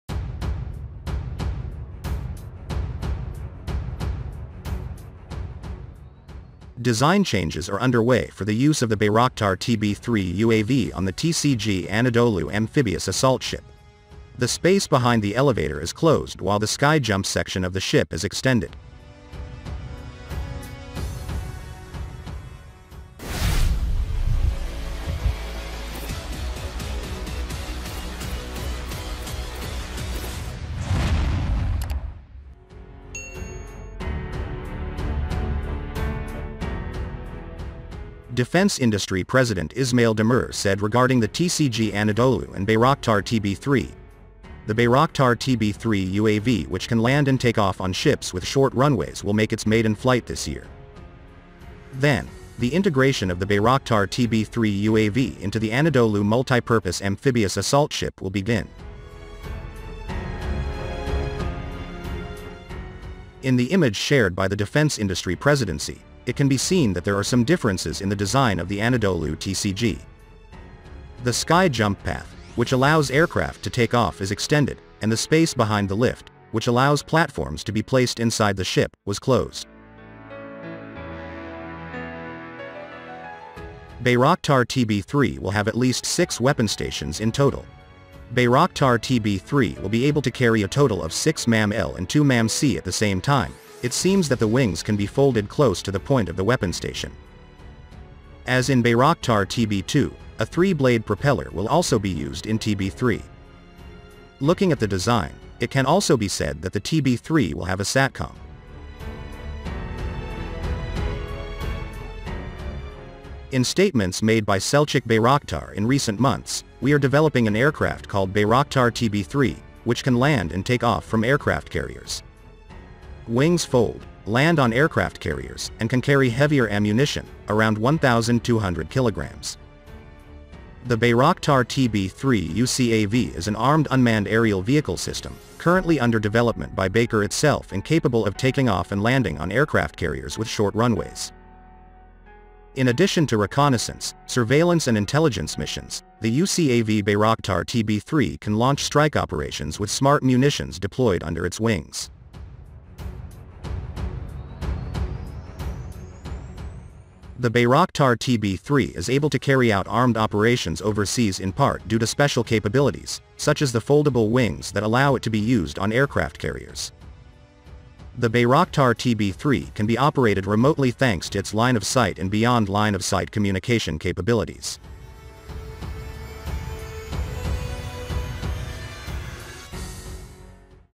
Turkish Changes Design of TCG Anadolu for the Use New Bayraktar TB3 UAVs
Design changes are underway for the use of the Bayraktar TB3 UAV on the TCG Anadolu Amphibious Assault Ship. The space behind the elevator is closed while the Sky Jump section of the ship is extended.